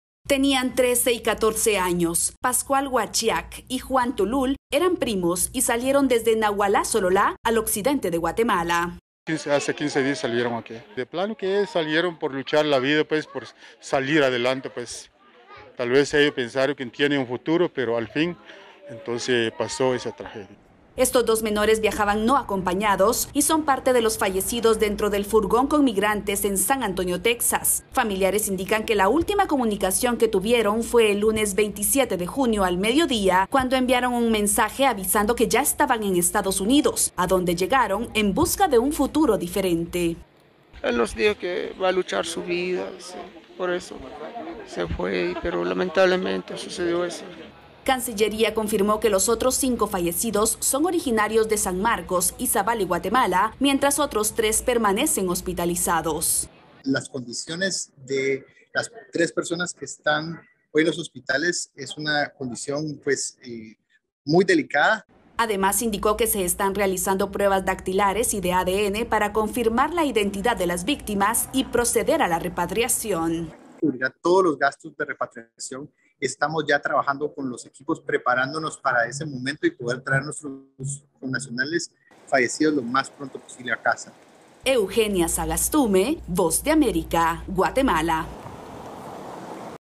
Menores no acompañados entre las víctimas de San Antonio
De los siete guatemaltecos víctimas de la tragedia de San Antonio, dos eran adolescentes y viajaban sin un adulto que los acompañara. Familiares aseguran que los dos menores migraron por razones económicas. Eugenia Sagastume tiene el relato desde Guatemala.

¡Visítenos!